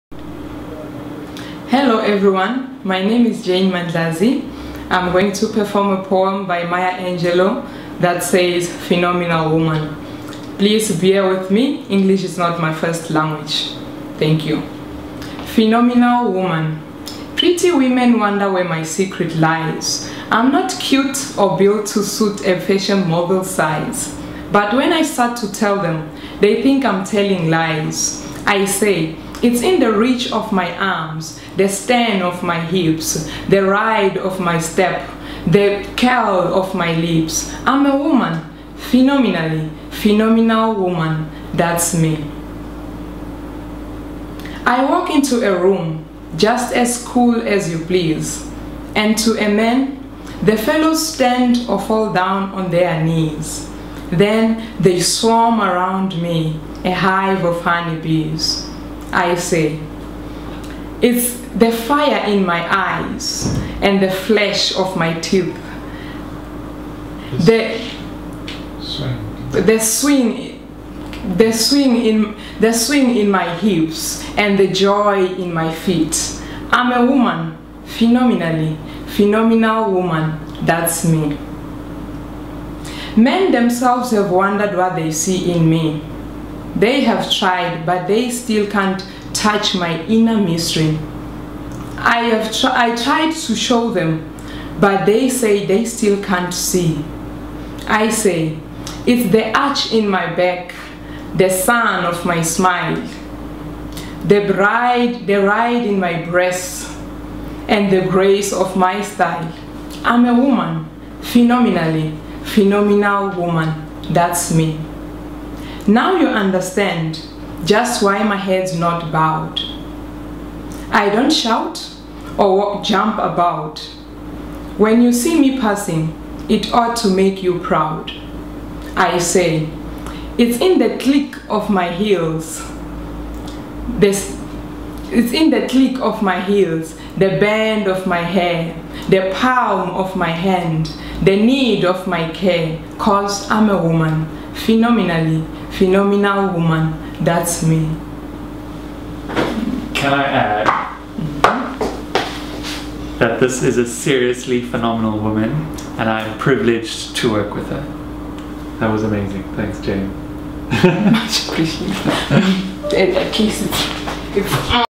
Jane Mandlazi Performs Maya Angelou's "Phenomenal Woman"
We're speaking out and we're taking inspiration from the beautiful words of Maya Angelou.